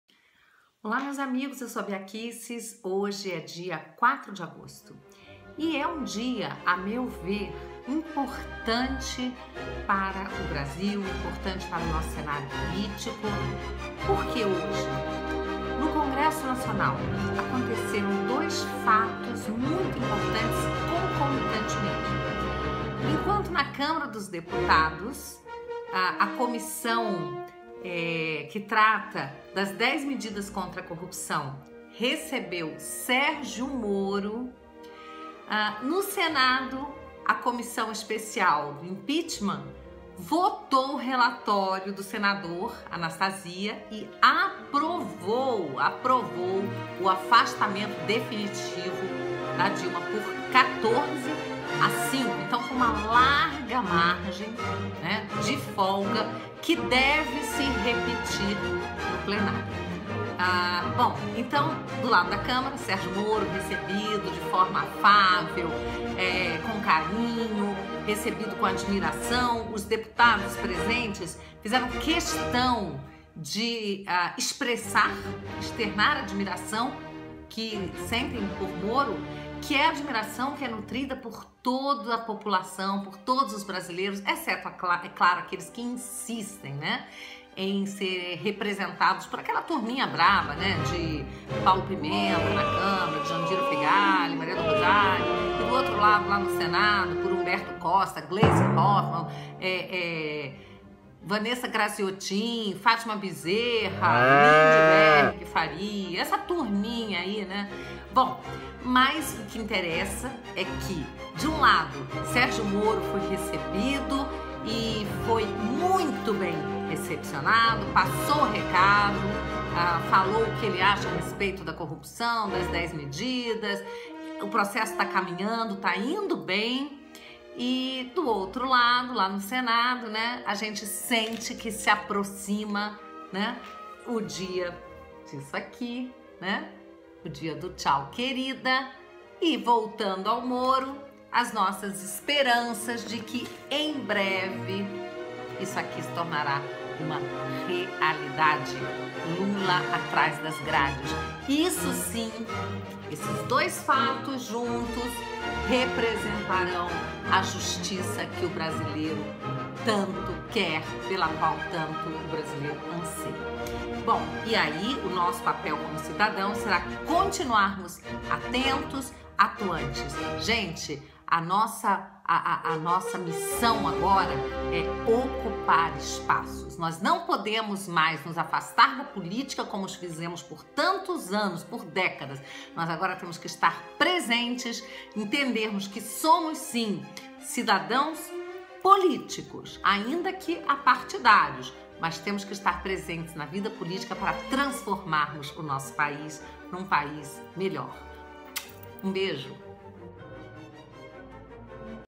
CEI aprova Impeachment e Moro fala sobre as 10 medidas. Dia quente no CN!
Hoje, 4/8/16 é um importante dia para os rumos da política nacional. Dois importantes eventos ocorreram no Congresso Nacional. Na Câmara, Moro se fez presente na audiência das 10 medidas e impressionou com seus argumentos. No Senado, a Comissão Especial do impeachment aprovou o Relatório do senador Anastacia com folga. Assista, curta e compartilhe!